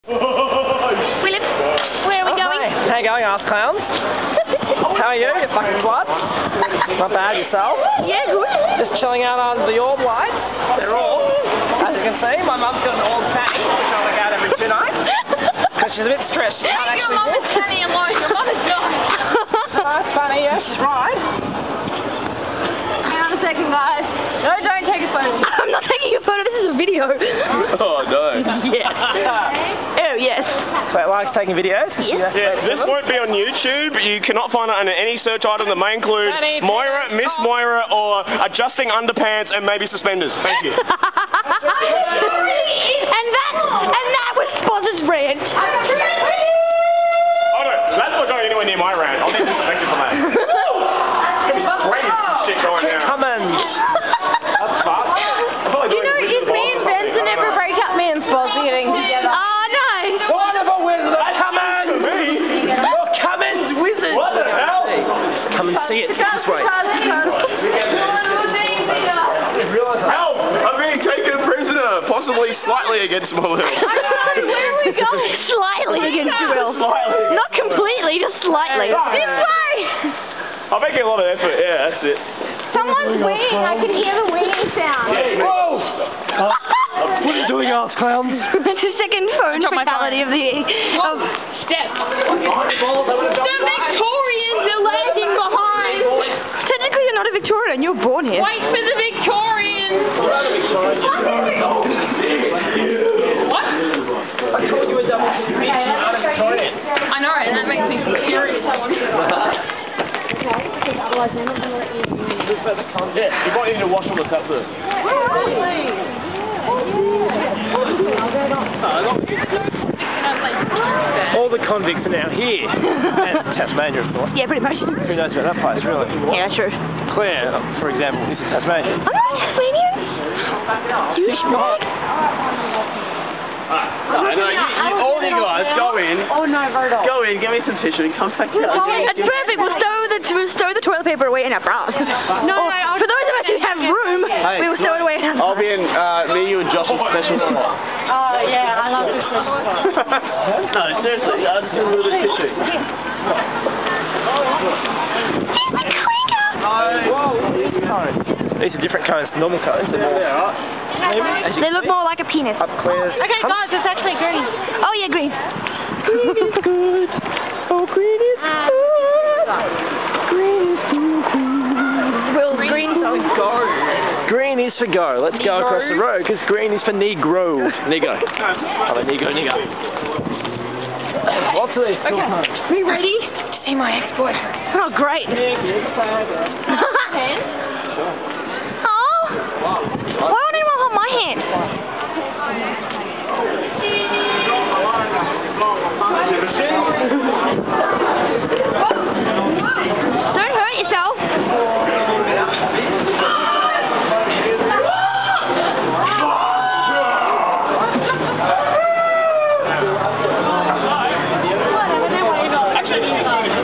Adelaide Antics Pt. 1 - The Walk.
Alice, Will, Moira, Mr. Joe, Miss Simone, Spoz and myself on a brisk walk through Adelaide towards the Cranka.